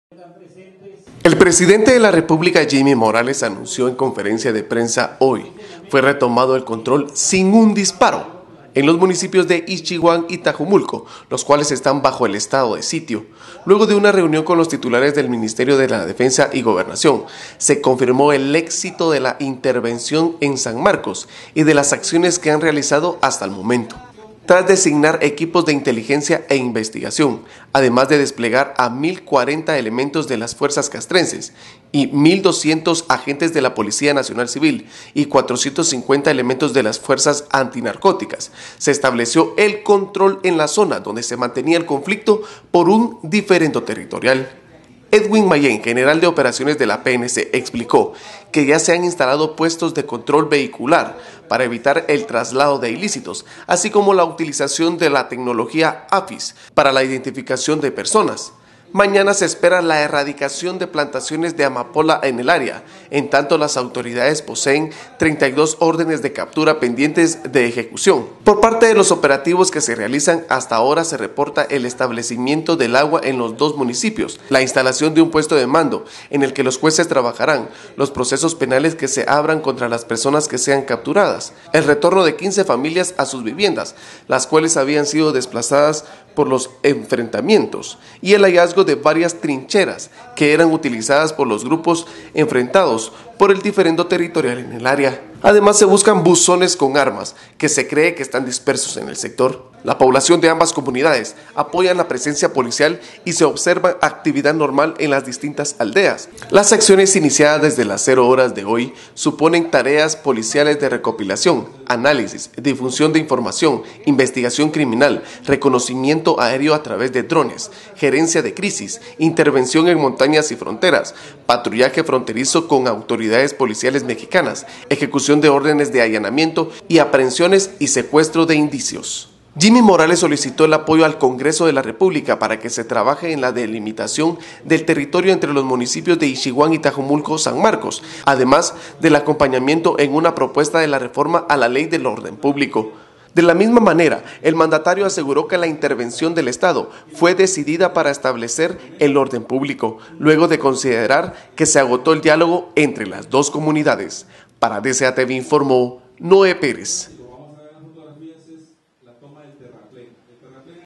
Retoman control en Ixchiguán y Tajumultco "sin un solo disparo"
El presidente de la República, Jimmy Morales, anunció en conferencia de prensa que hoy fue retomado el control, "sin un solo disparo", en los municipios de Ixchiguán y Tajumulco, los cuales están bajo estado de sitio.
Luego de una reunión con los titulares de los ministerios de Defensa y Gobernación, se confirmó el éxito de la intervención en San Marcos y de las acciones que se han realizado hasta el momento.
Tras designar equipos de inteligencia e investigación, además de desplegar a mil 40 elementos de las fuerzas castrenses, mil 200 agentes de la Policía Nacional Civil (PNC) y 450 elementos de las fuerzas antinarcóticas, se restableció el control en la zona donde se mantenía el conflicto por un diferendo territorial.
Edwin Mayen, general de operaciones de la PNC, explicó que se han instalado puestos de control vehicular para evitar el traslado de ilícitos, así como la utilización de la tecnología AFIS para la identificación de personas.
Mañana se espera erradicar plantaciones de amapola en el área, en tanto las autoridades poseen 32 órdenes de captura pendientes de ejecución.
Acciones exitosas
Como parte de los operativos que se realizan, hasta ahora se reporta el restablecimiento del agua en los dos municipios, las instalación de un puesto de mando en el que jueces trabajaran los procesos penales que se abran contra las personas que sean capturadas, el retorno de 15 familias a sus viviendas (las cuales habían sido desplazadas por los enfrentamientos) y el hallazgo de varias trincheras que eran utilizadas por los grupos enfrentados por el diferendo territorial en el área.
Además, se buscan buzones con armas, que se cree están dispersos en el sector.
“Entre los resultados positivos está la inexistencia de disparos en el área, la colocación de patrullajes y puestos de control que han traído la calma a la población”, destacó Francisco Rivas, ministro de Gobernación.
La población de ambas comunidades "apoya la presencia policial y se observan actividades normales en las distintas aldeas", apuntó la PNC en un tuit.
Las acciones iniciadas desde las 00:00 horas de hoy suponen tareas policiales de recopilación, análisis y difusión de información, investigación criminal, reconocimientos aéreos a través de drones, gerencia de crisis, intervención en montañas y fronteras, patrullajes fronterizos con autoridades policiales mexicanas, ejecución de órdenes de allanamiento y aprehensión y secuestro de indicios.
Proponen resolver los límites
Jimmy Morales solicitó el apoyo del Congreso de la República para que se trabaje en la delimitación del territorio entre los municipios de Ixchiguán y Tajumulco, San Marcos. Además del acompañamiento en una propuesta de reformas a la Ley del Orden Público.
De la misma manera, el Mandatario aseguró que la intervención del Estado “fue decidida para restablecer el orden público”, luego de considerar “agotado el diálogo entre las dos comunidades".